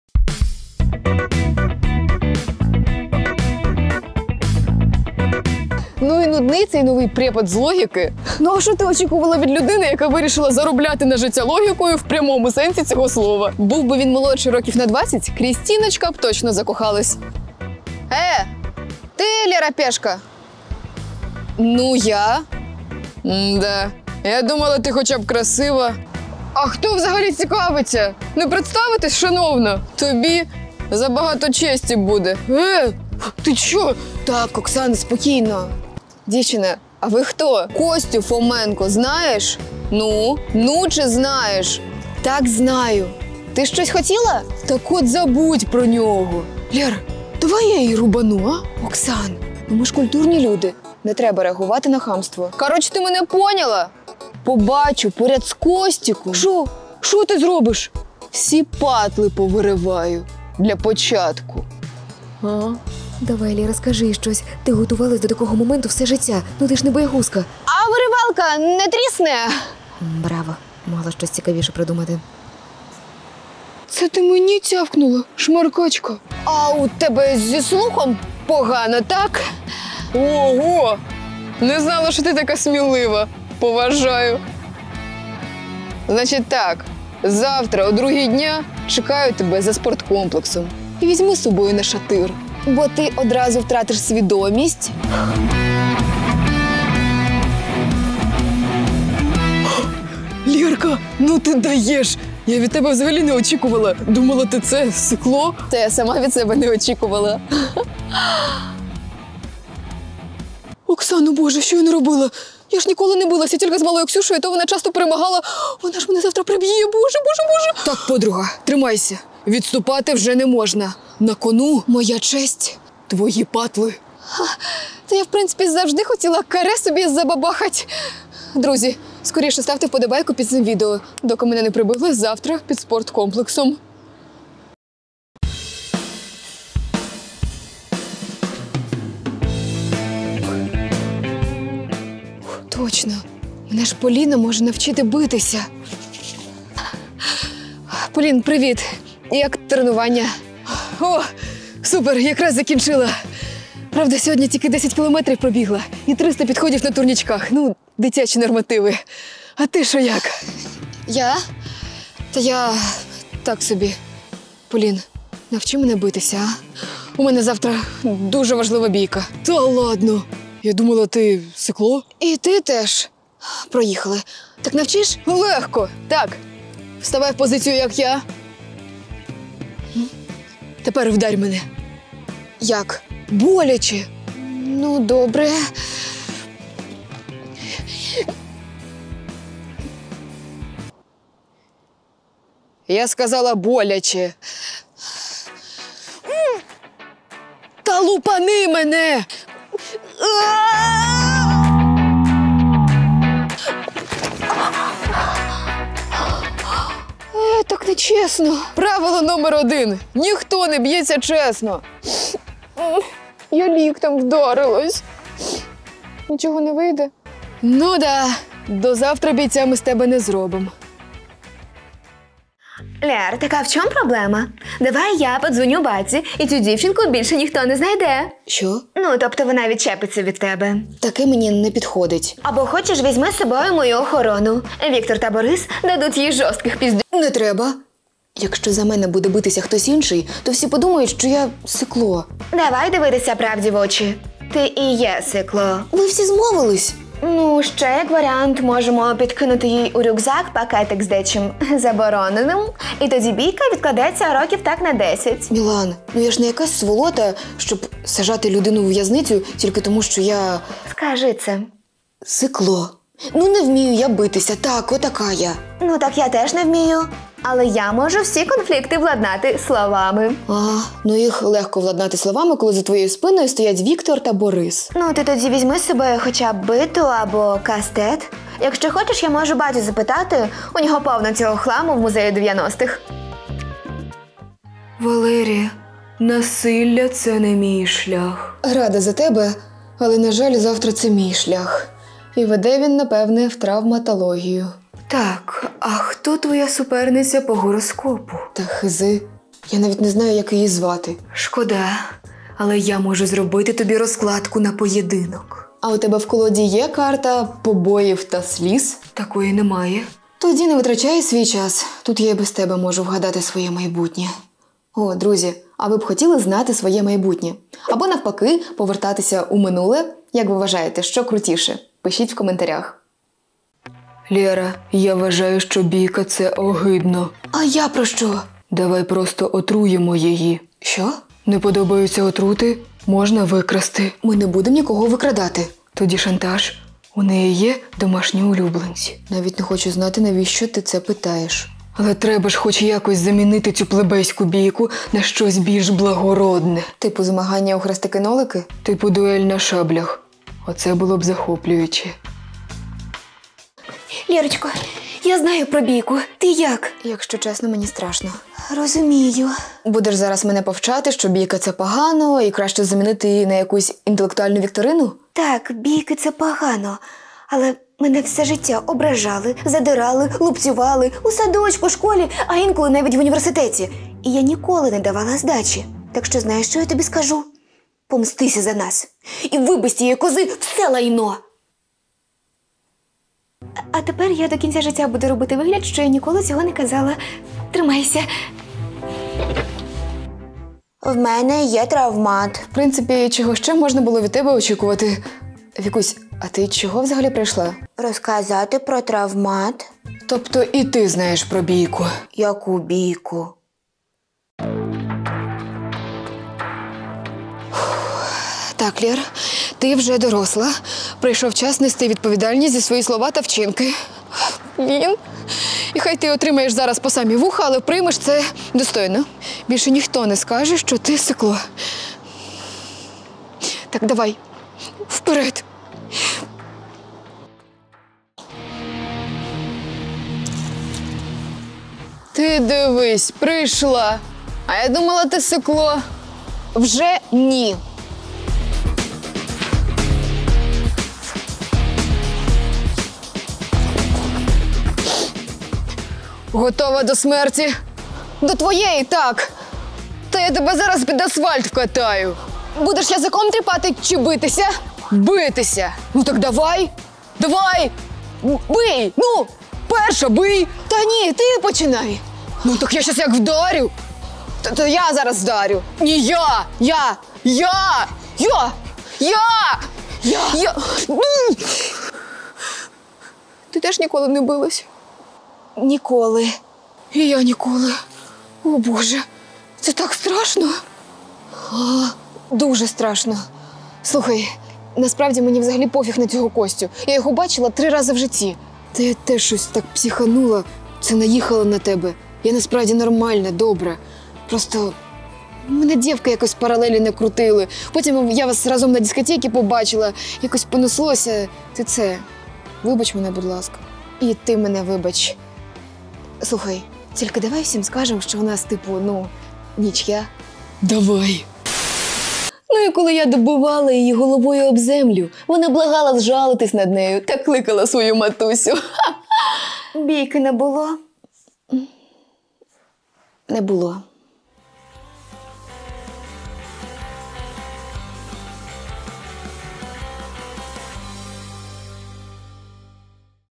МОЯ ПЕРША БІЙКА. Пешка
Сьогодні буде перша бійка Лери, хто знає чим все скінчилося, як би не… коротше давіться самі!